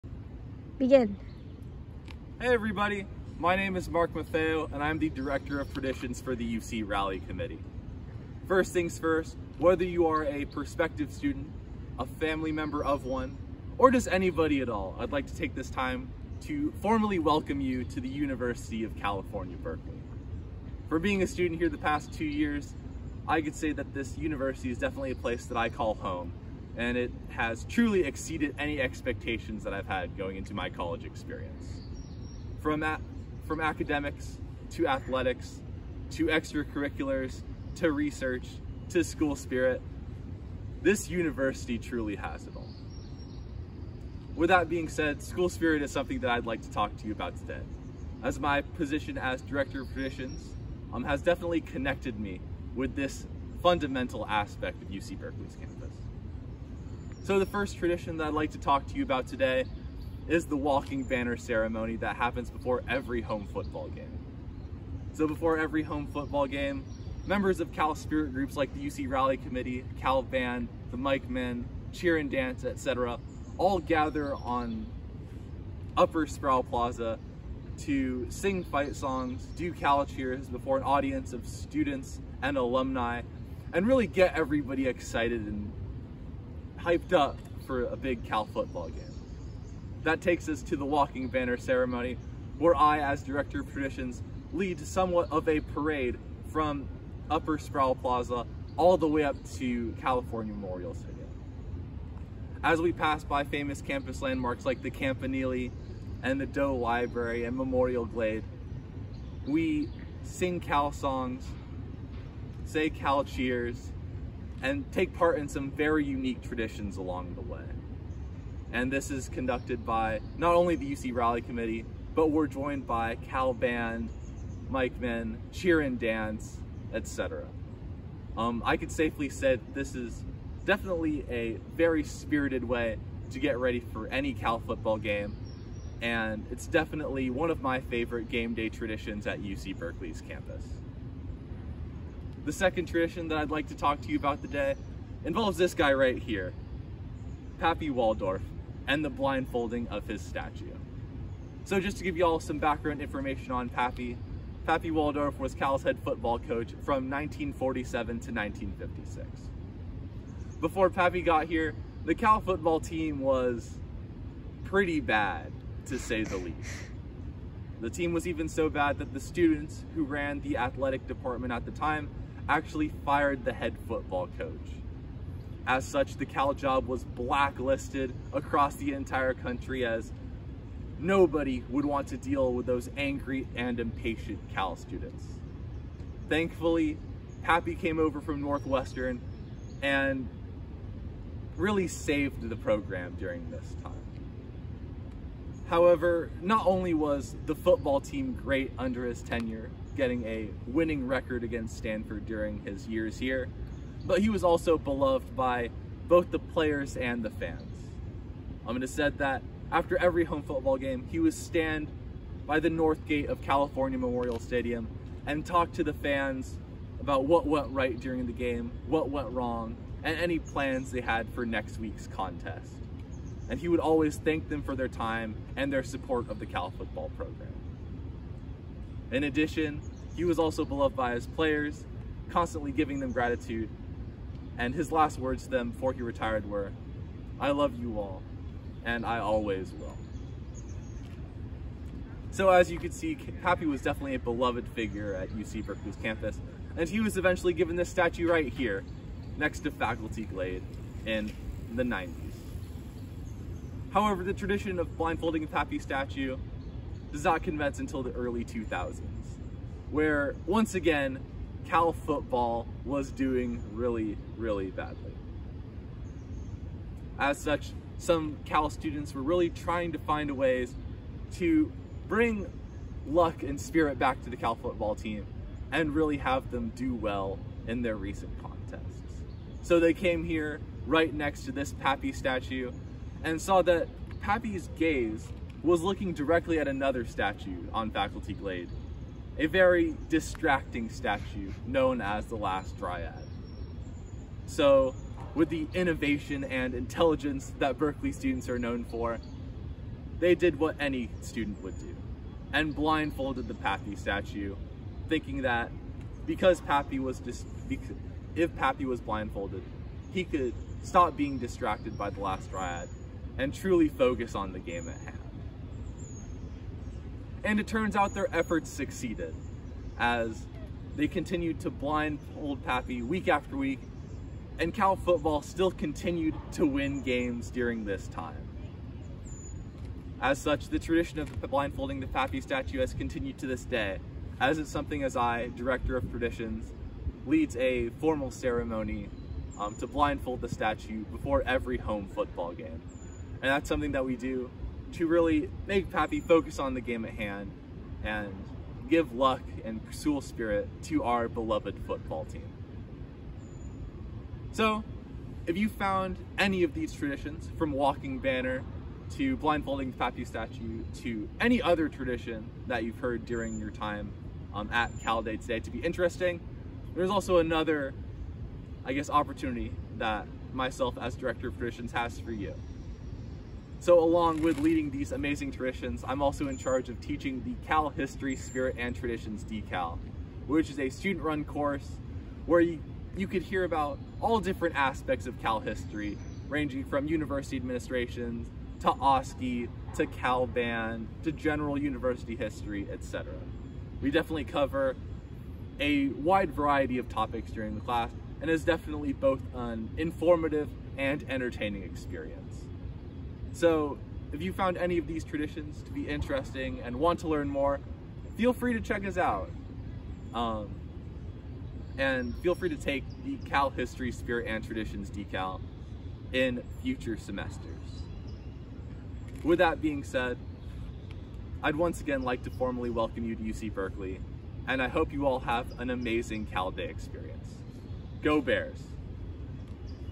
Cal Day 2021 - Director of Traditions
Find out how the Director of Traditions spreads Cal Spirit!